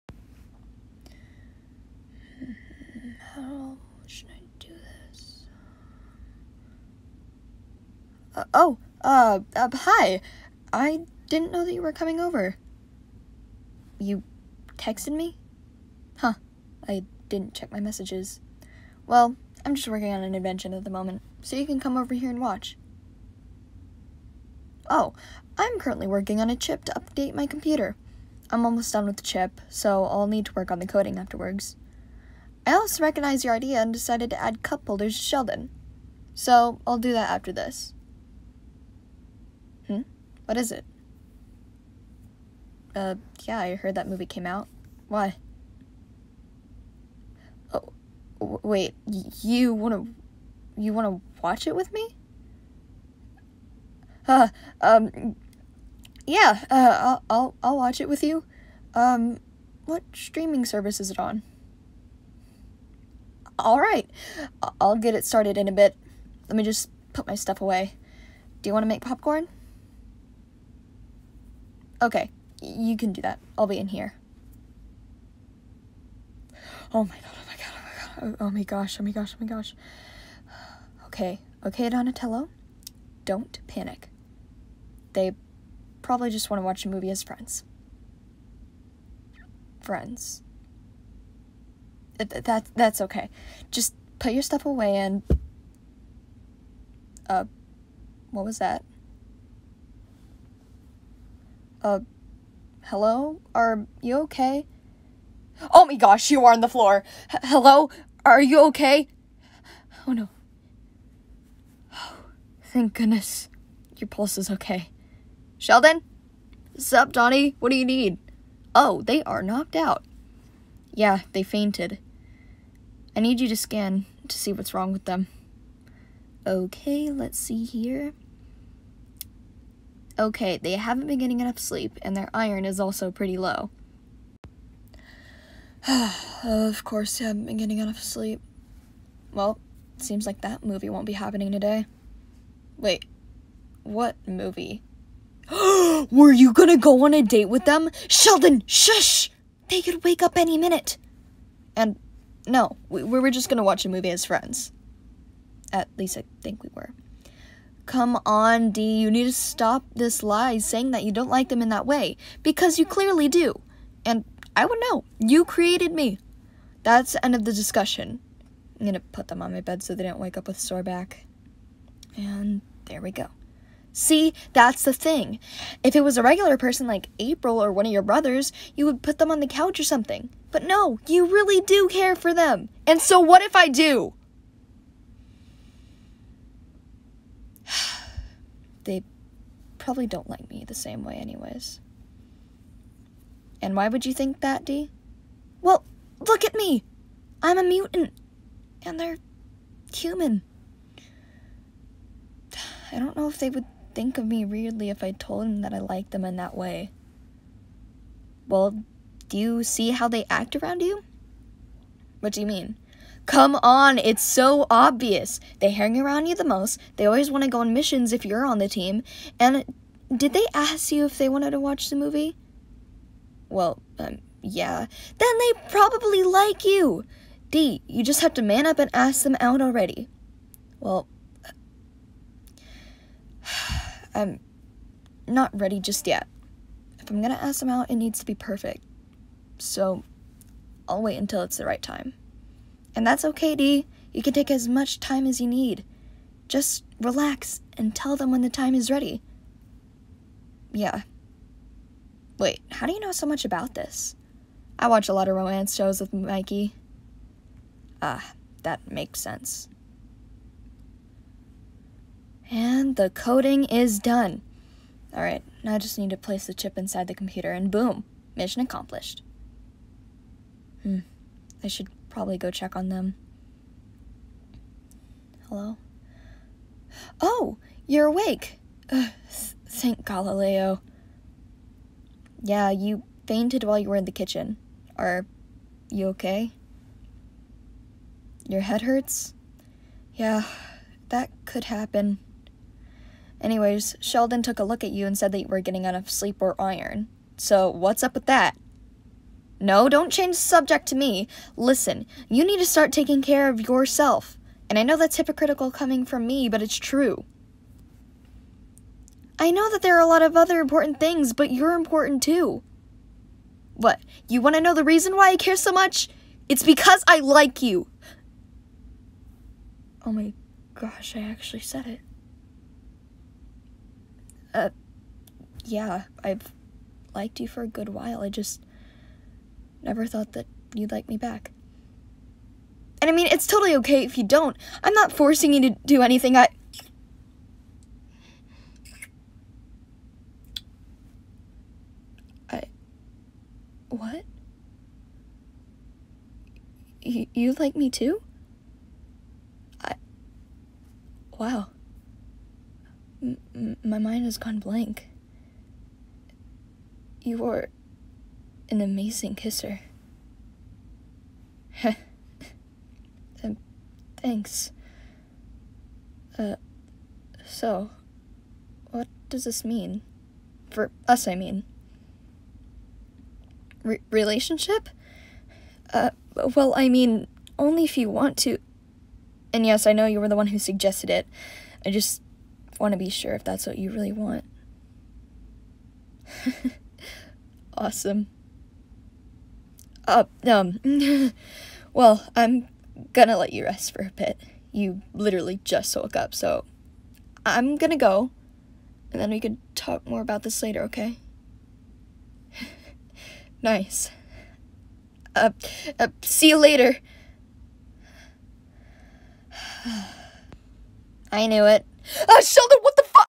You faint and Donnie takes care of you | Donnie x Listener Audio
Hello everyone!
I know I literally just posted like a day ago but @yukiva1822  gave me the GREATEST IDEA for an audio and I couldn't get it out of my head.
So I created the script, drew the thumbnail, and recorded myself in front of my phone's mic.
Thank you Yuki for giving me an idea for a video!
From now on I'm gonna be taking Donnie x Listener requests because brainstorming takes a while.
Well, I'll see you guys in the video! Bye!